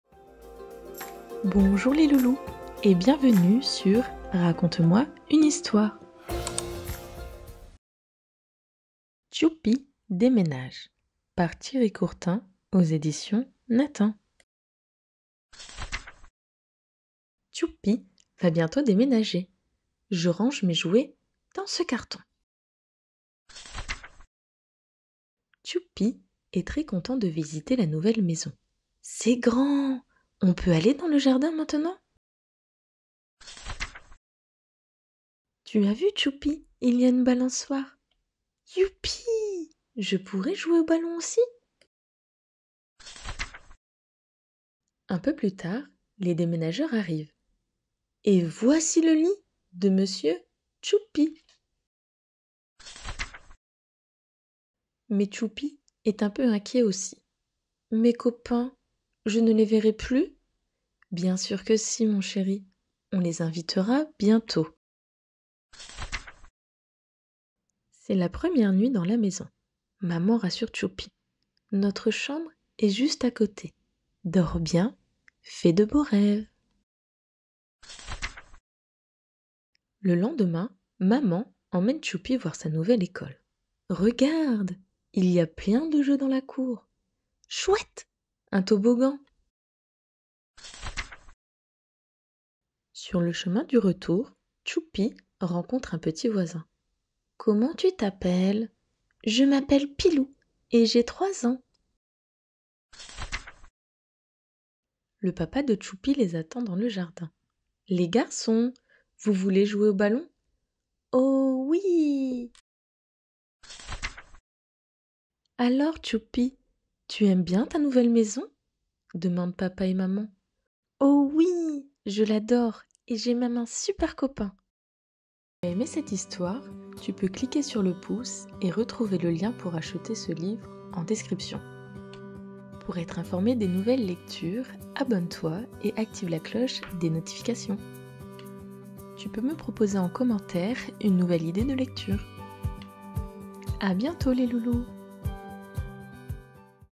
Lecture : T'choupi déménage
Lecture de livres pour bébé & enfant

Titre : T'choupi déménage
Édition : Nathan
Auteur : Thierry Courtin

Résumé : T'choupi déménage dans une nouvelle maison. Il est très excité mais aussi un peu inquiet...

Accompagné son enfant lors d'un déménagement, le rassurer, lui expliquer ce qu'il va se passer.

Tu peux acheter ou offrir ce livre